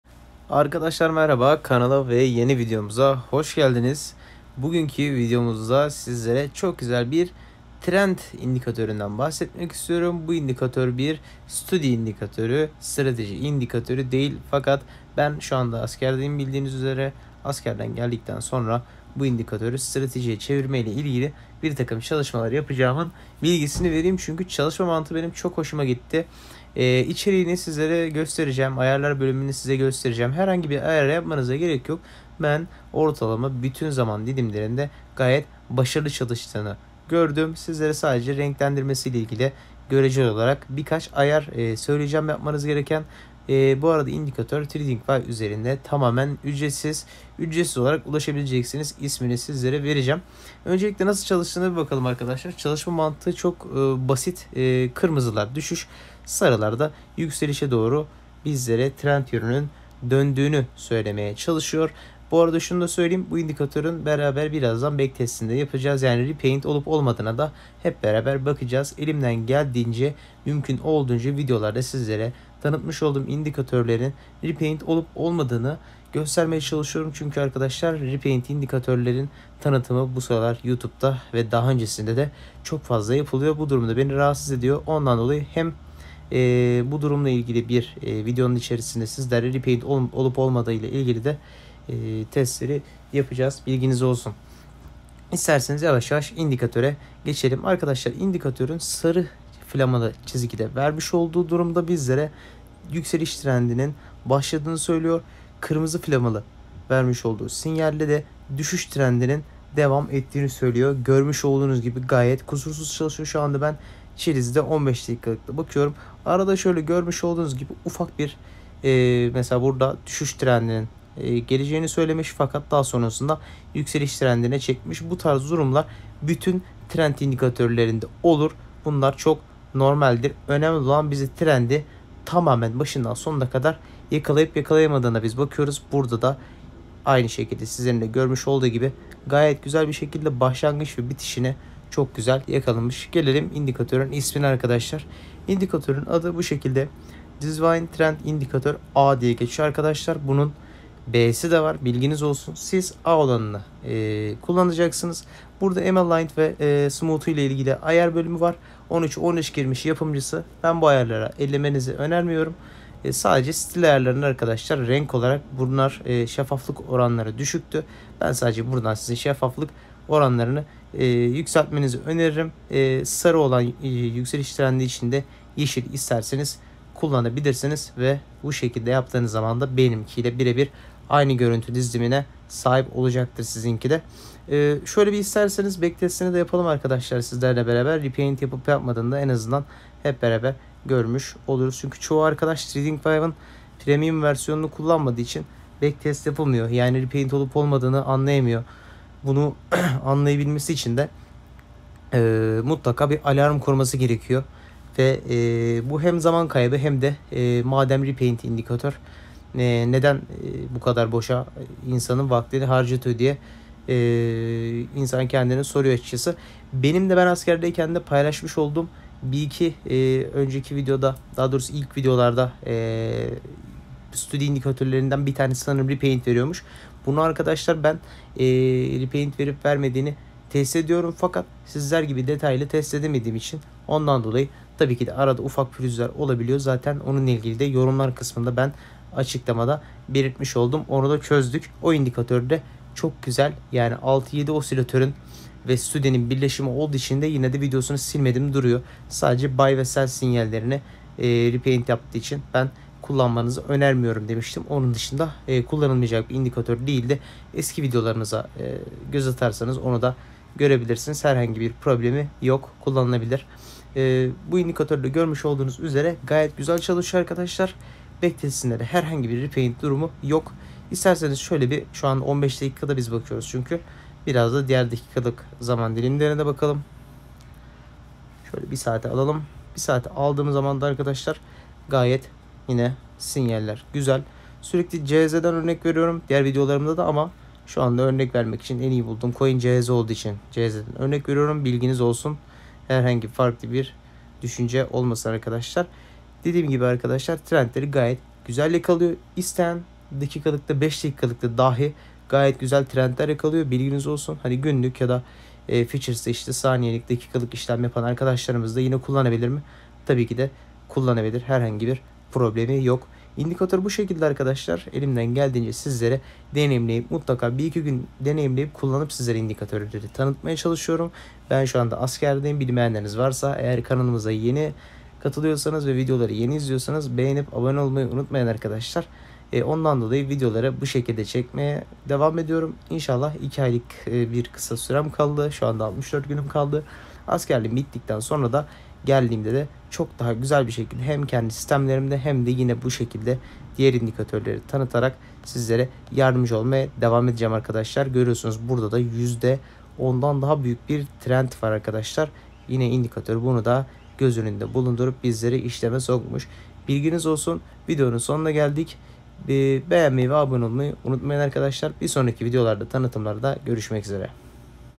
TRENDİ DOLU DOLU YAKALAMAK!
indikatör

Merhaba Arkadaşlar,

Bu gün sizlere şahane bir trend indikatörü tanıttım gerçekten içime sinen bir indikatör olduğunu bilmenizi isterim ve beğendiyseniz indikatörü videoyu beğenmeyi unutmayın bol kazançlar..

Udemy Eğitimi ve içerisinde 3 Ay hediyeli Rolinlong indikatörleri;